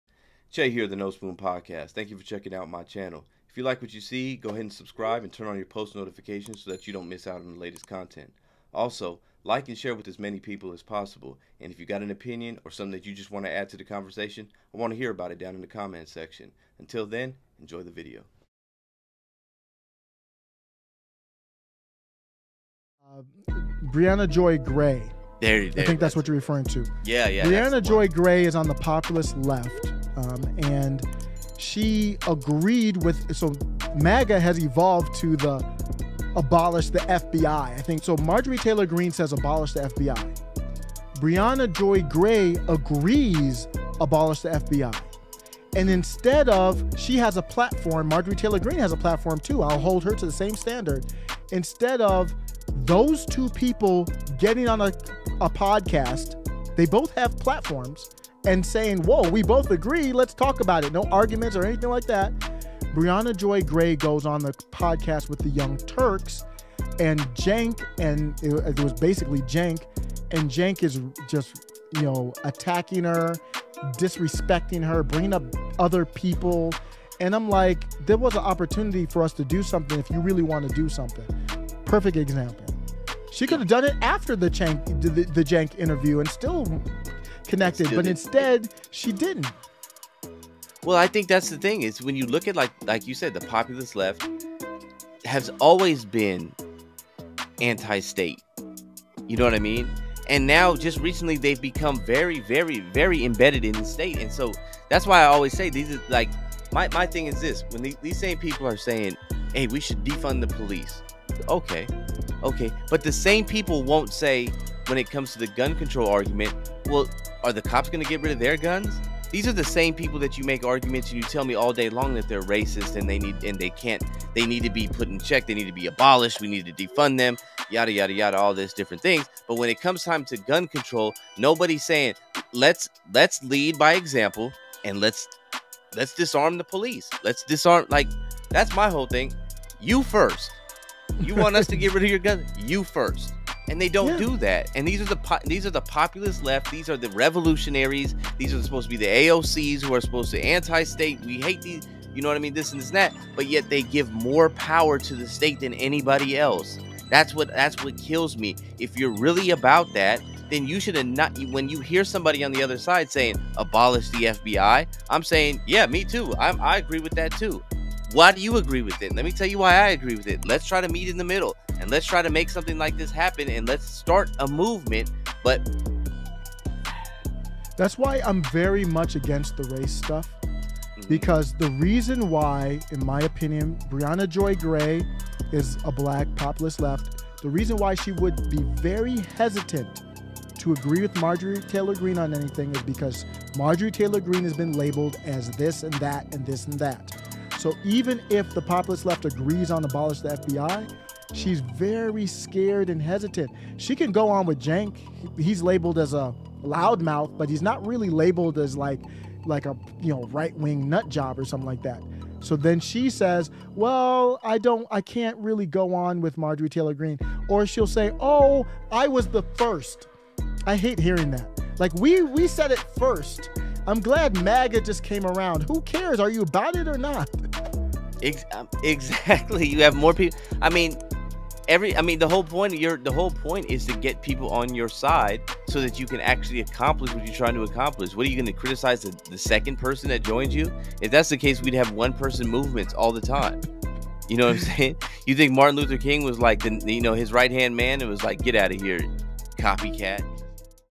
Uniting the Right and Left w/ Osiris of
The polarization of American politics between left and right seem almost irreconcilable.  But, there are points where the two sides do agree.  How do we use these opportunities to unite the political sides and bring unity against the real enemies of the people?